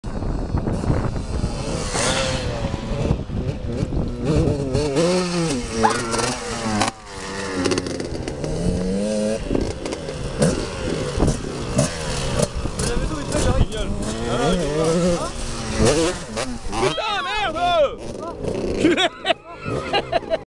premier saut nico et sa nouvelle RM .....
rodage de nico et le pauvre dim tout sale !